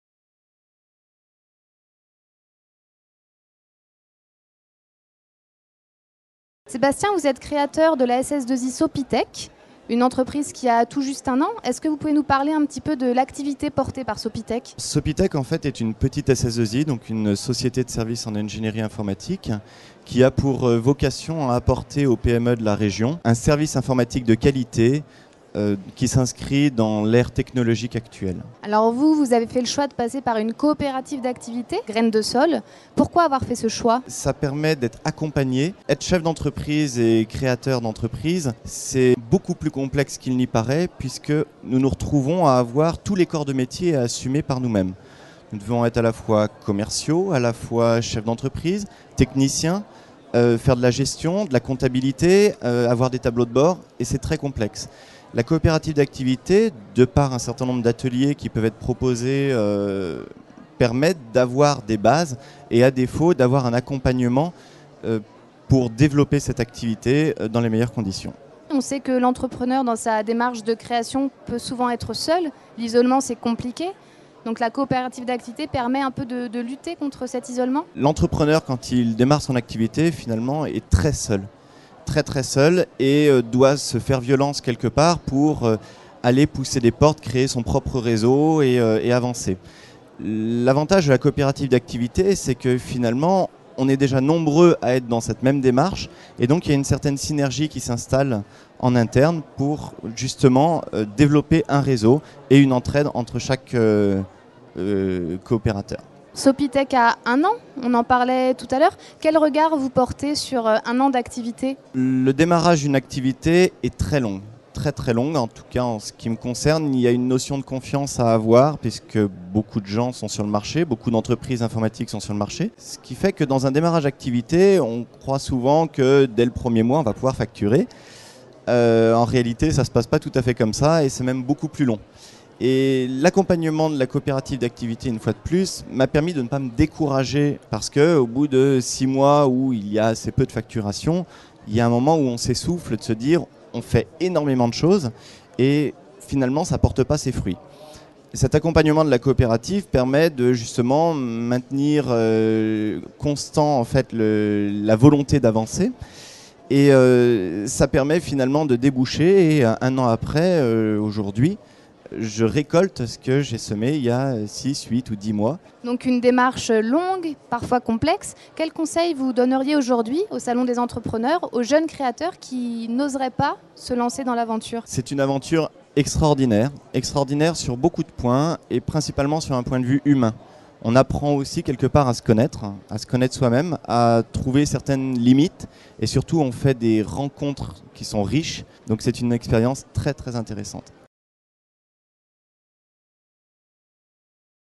La coopérative d'activités vue par Sopitec
Lorsque Sébastien Bardet-Massin crée Sopitec en 2011, il opte pour le statut d'entrepreneur-salarié, au sein de la  coopérative d'activités lyonnaise Graines de SOL. Un an après la création de la SSII, il  revient sur les raisons de son choix.